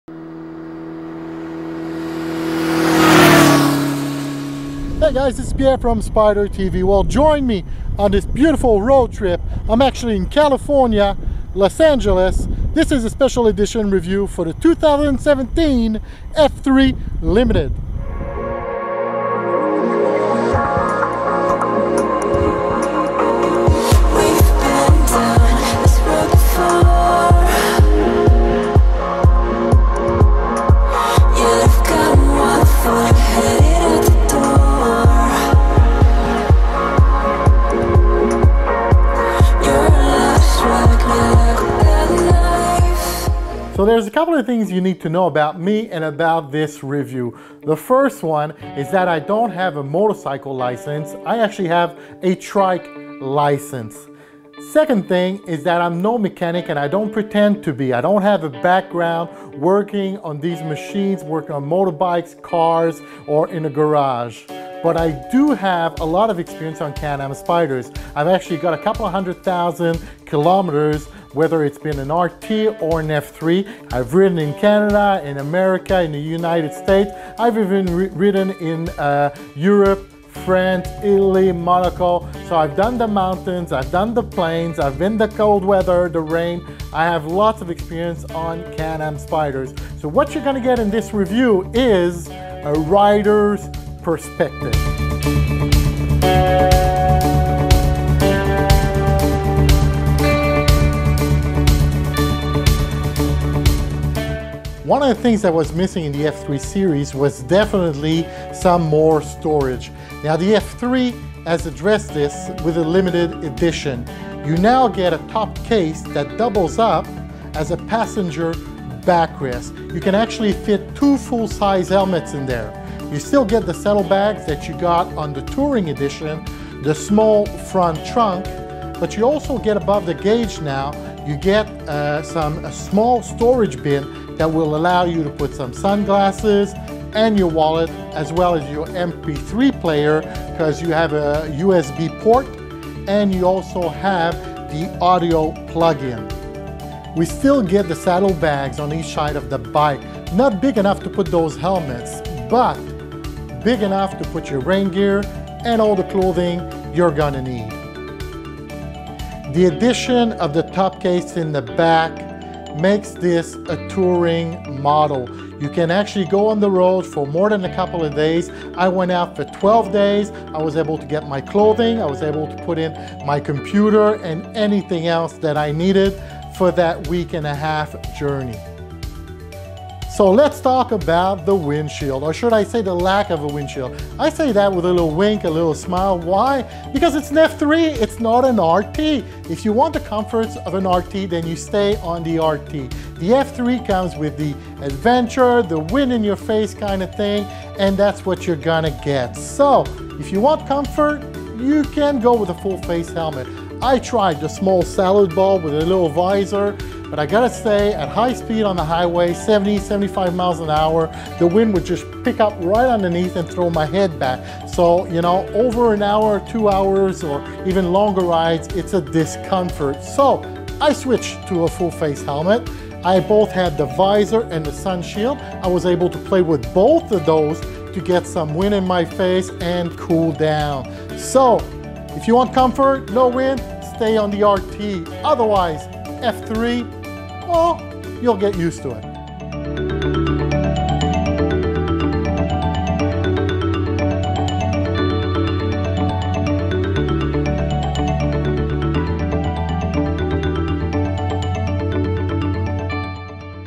PART 1 of 3 - 2017 F3 Limited Can Am Spyder Review - 10 day Roadtest
In this video i'm sharing my 10 day road test and review of this 2017 F3 limited Can Am Spyder. From San Diego to San Jose along the coast, i put on over 2000 miles in 10 days. Find out the good and the. not so good about this bike.

Pierre Poliquin - Spyder TV on Youtube.